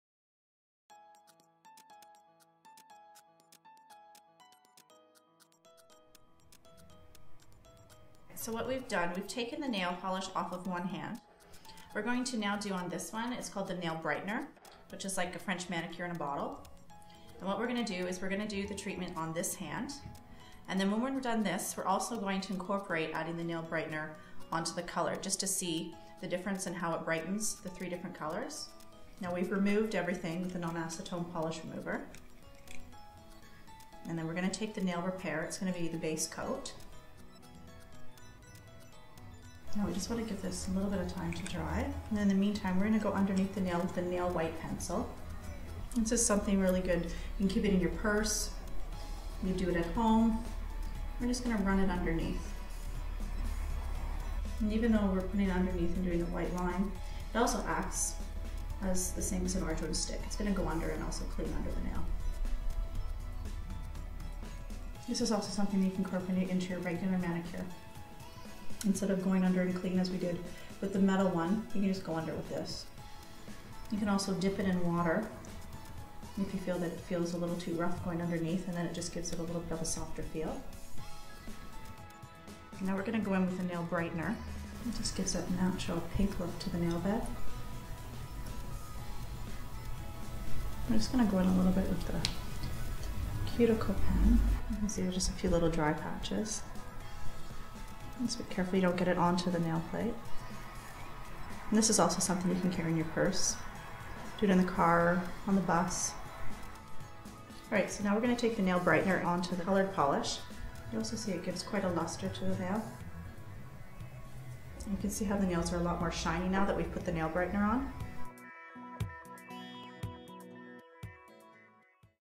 Nail Brightener & White Pencil - Instructional Video
The 30-second French Manicure in a bottle — it's no wonder Trind Nail Brightener is one of our top sellers.

Trind Nail White Pencil temporarily enhances the whiteness of the nail tip and works well in conjunction with Trind Nail Brightener for a 30-second French Manicure!